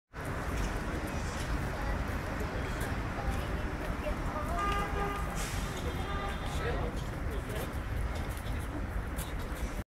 University Square
One of the central places of Bucharest. You can see the University of Bucharest and I hope you like it.